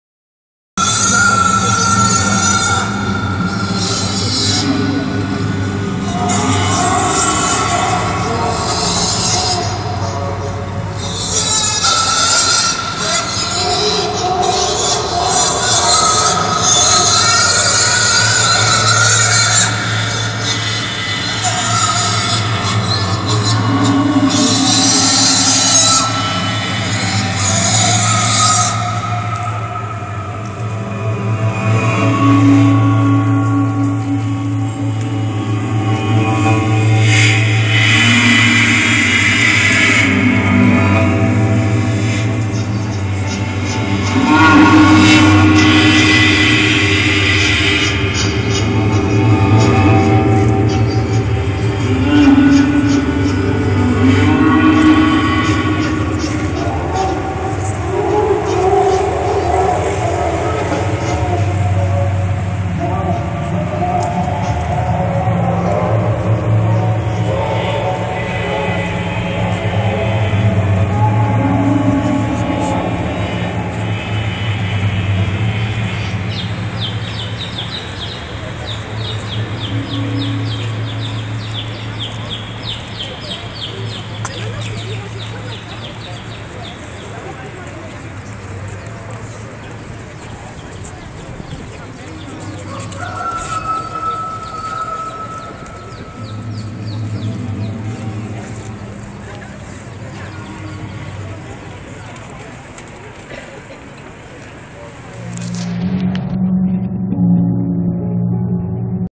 Paris 26/09/15 HAPPENING MOUVEMENT 269 LIFEFRANCE
C'est sur la place du Palais Royal qu'a eu lieu cet Happening 269 Life France afin de mettre en avant l'éthique animale et montrer la souffrance des animaux et leur cruelle exploitation.